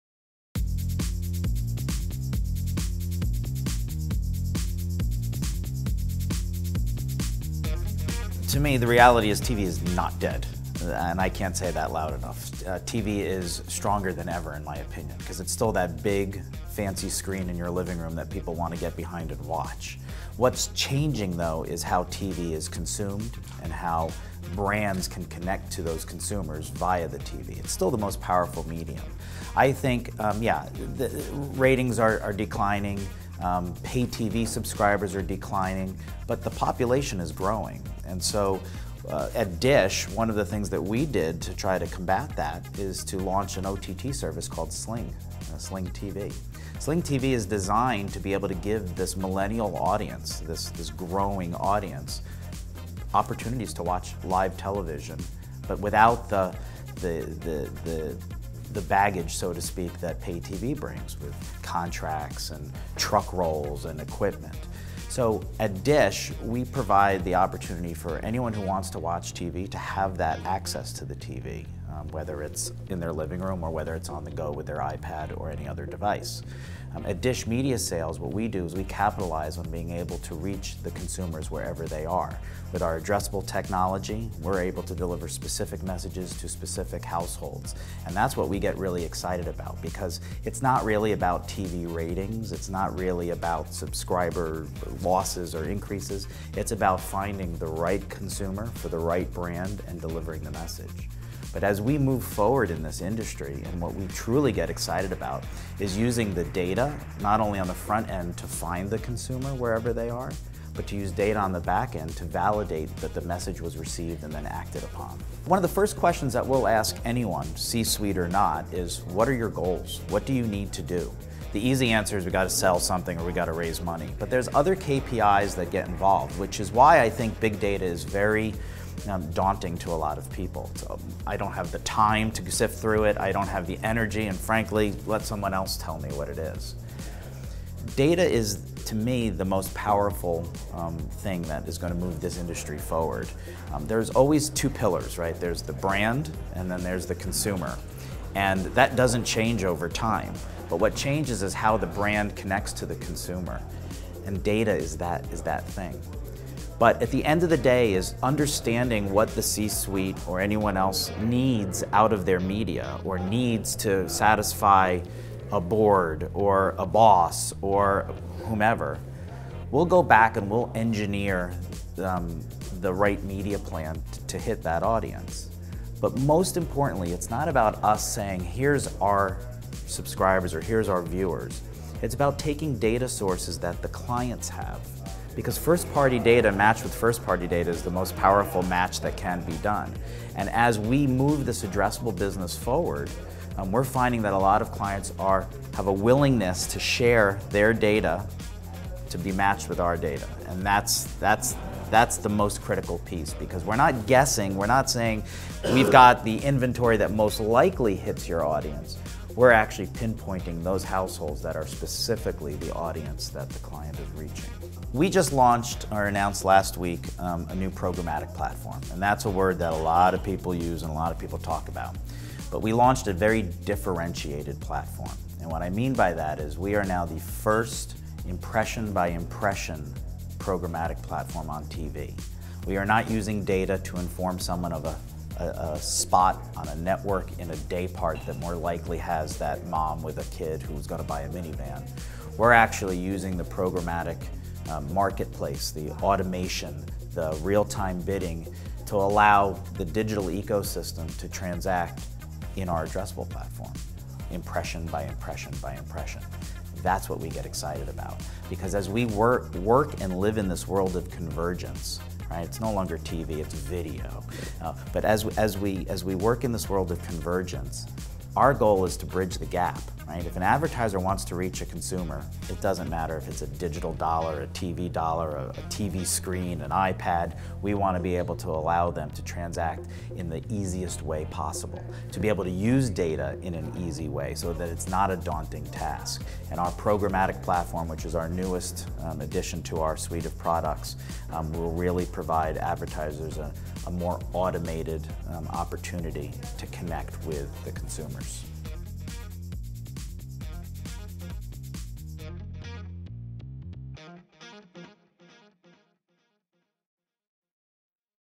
DISH TV: Ratings Might Be Down But ROI is Going Up - Adam Gaynor
"Ratings are in decline. But, TV is still the most powerful medium. TV is stronger than ever," says Mr. Gaynor. Might leave one scratching their head. How can that be? In this interview, Adam describes how the TV industry, and Dish specifically, is adapting the way TV is delivered to meet the needs of today’s mobile consumer and provide advertisers with data-driven, addressable opportunities that allow them to find the right consumer at the right time. Mr. Gaynor is excited about the future of TV and particularly how big data is providing more efficiency for TV advertisers. "It's not only using data on the front end to find the right consumer wherever they are, but on the back end to validate the message was received and acted on." That may be the promise of TV of the future, but how accessible is it today? Check out Adam's comments about video conversion and the opportunities of leveraging data for video investments. It's a real peek into the future of the medium and the future just might already be here.

Featuring:
Adam Gaynor, VP, Media Sales & Analytics, Dish